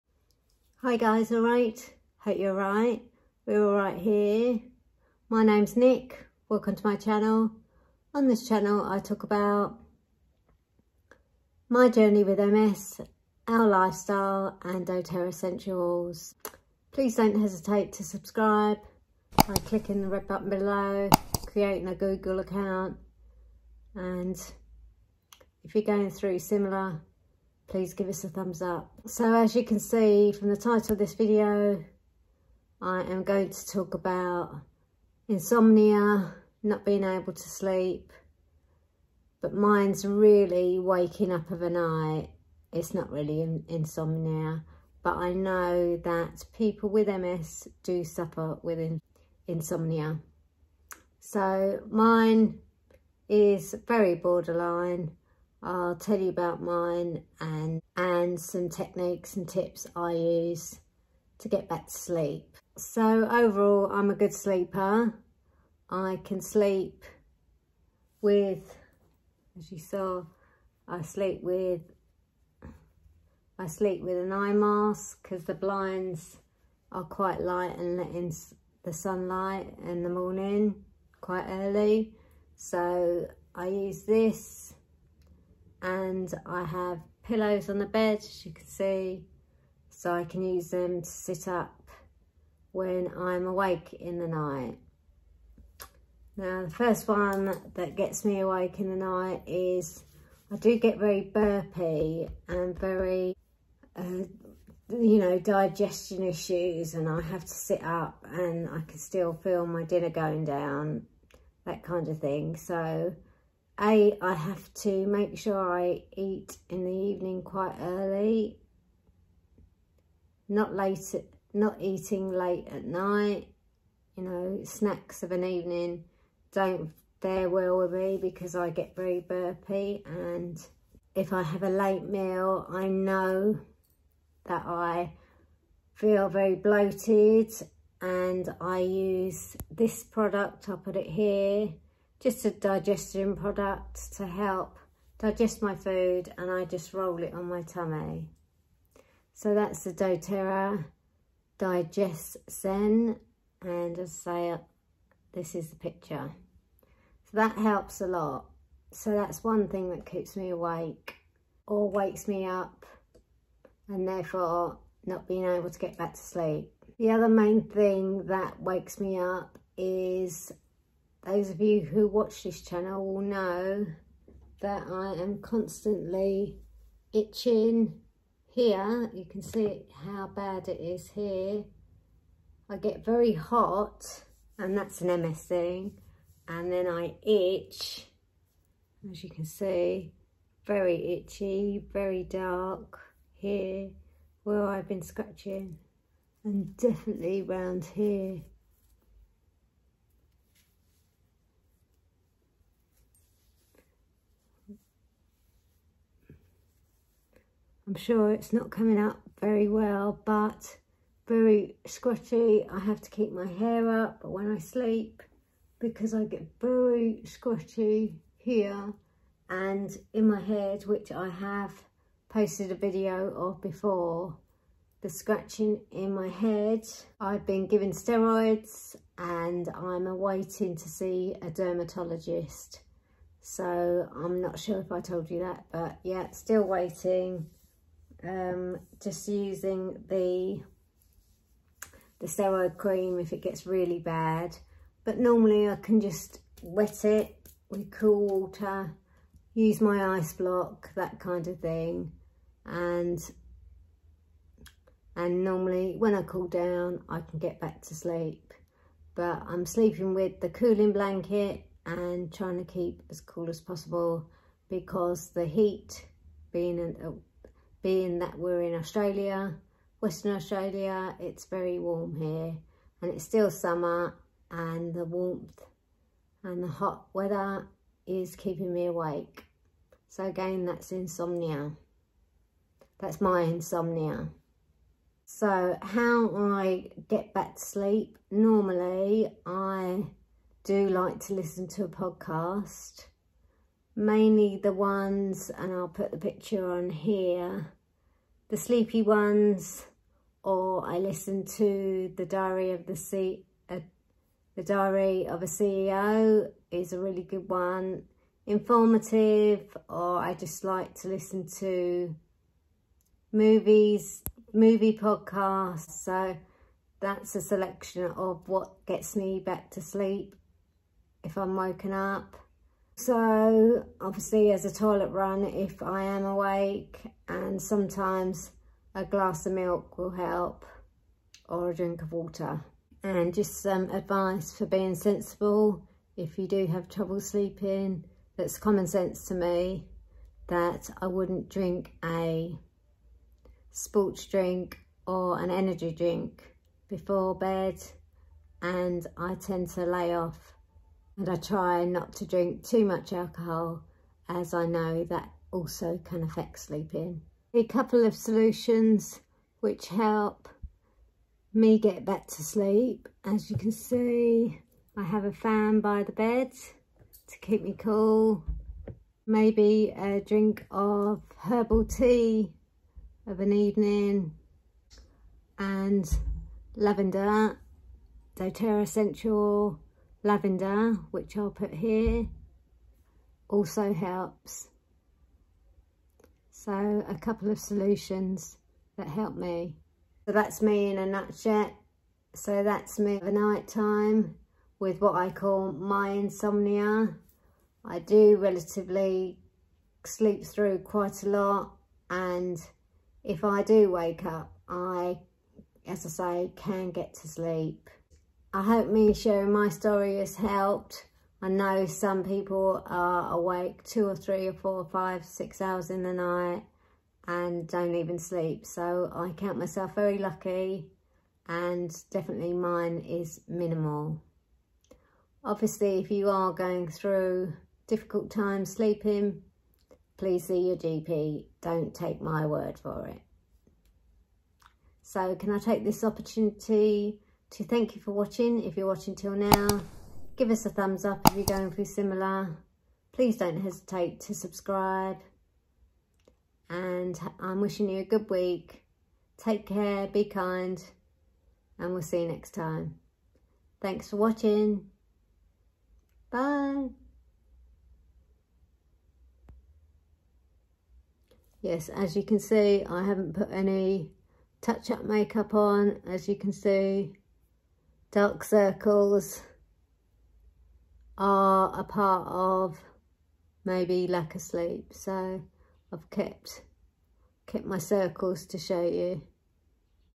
Insomnia with Multiple Sclerosis
In this video I talk about my what makes me wake in the night and how I get back to sleep.

My details are

FB page Alright Lifestyle with Nic

Instagram alright_lifestylewithnic / myfashuntreasure.